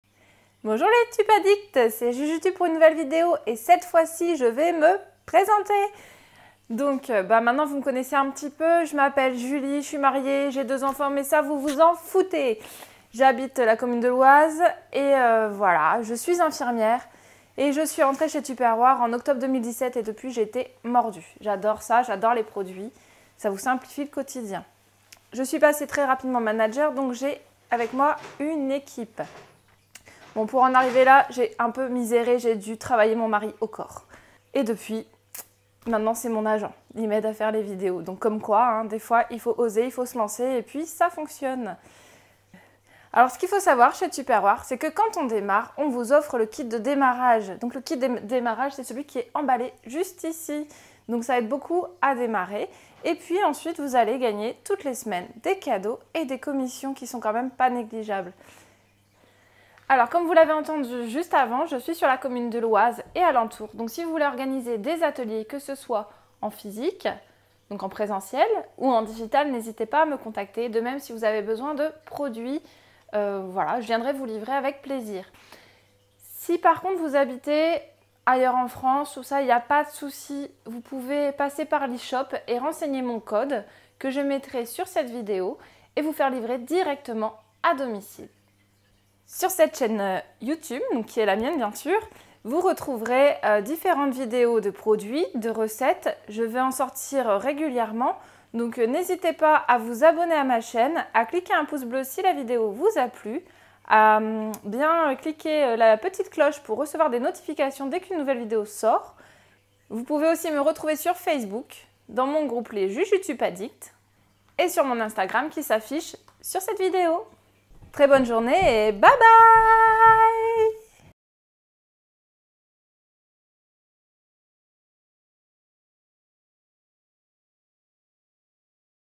Qui suis-je ? (ma présentation)
Je suis Julie, je suis mariée, j'ai 2 charmantes petites filles.
Je suis rentrée chez tupperware par hasard en octobre 2017, une amie animée mon atelier et elle m'a proposé l'activité.
Au bout des 4 semaine nous avons fait le point et c'était tout vu, cette activité me rend épanouie, j'ose m'exprimer et prend confiance e moi.
Très vite le confort financier se fait sentir et nous avançons dans les travaux de notre maison.
Je suis devenue manager au bout de 3 mois, j'ai une équipe qui déchire avec un objectif solidarité, sans prise de tête mais toujours présentes les unes pour les autres.

Tupperware & Eshop